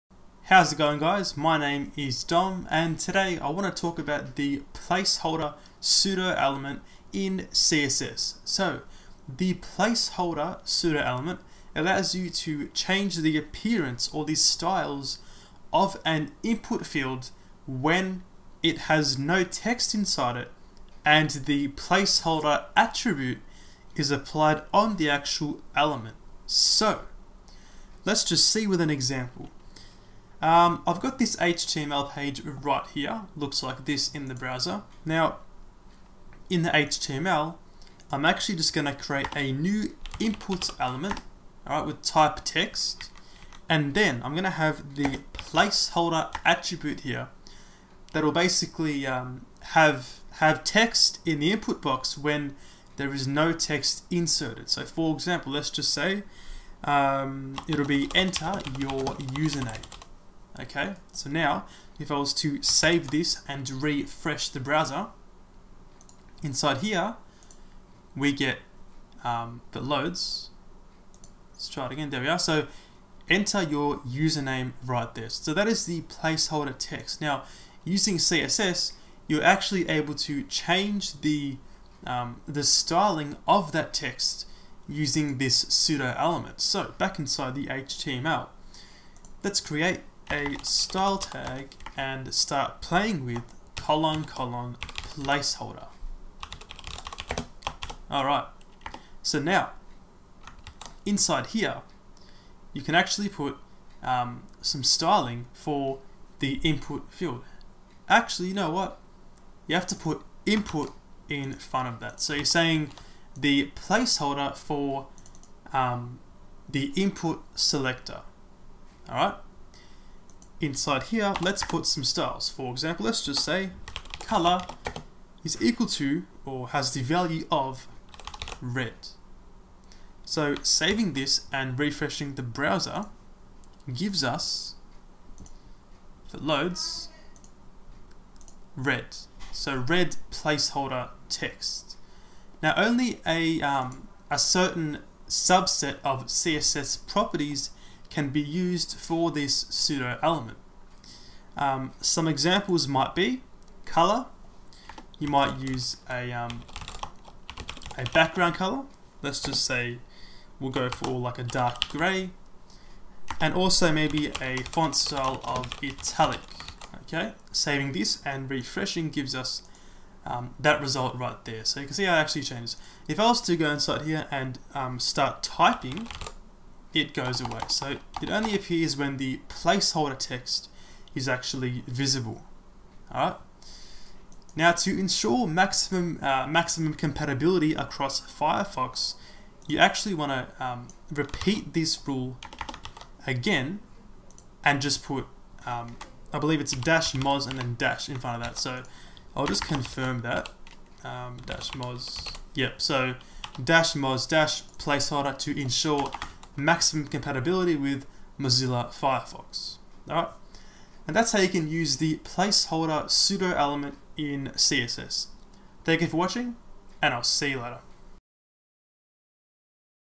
CSS Tutorial - ::placeholder pseudo-element | HTML Form Styling
By using the ::placeholder pseudo-element in CSS, you are able to change the styling (appearance) of the "placeholder text" you can find in HTML input forms.

Only a limited number of CSS rules are supported by the ::placeholder pseudo-element, which you can find here:

In this video I take you through a basic example of how to use ::placeholder which includes altering the appearance of an input field.